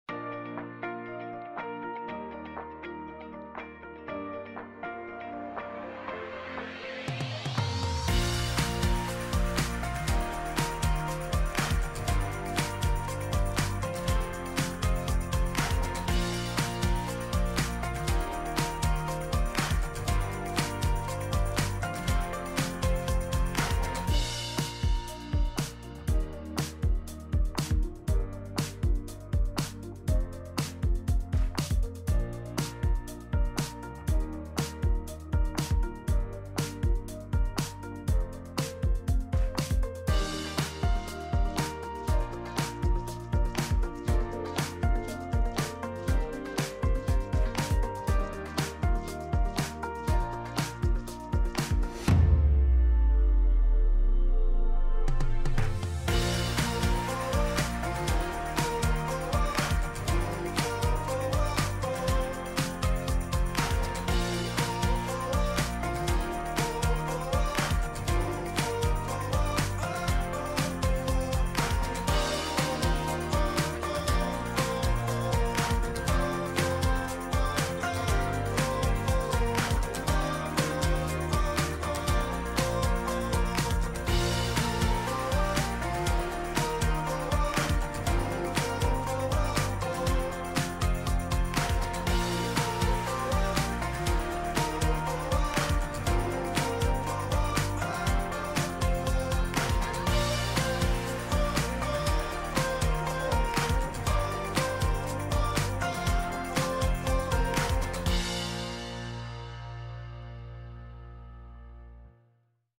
A Trip Around Livingston Campus | Rutgers–New Brunswick
Take a peek at Livingston Campus at Rutgers–New Brunswick in this short preview.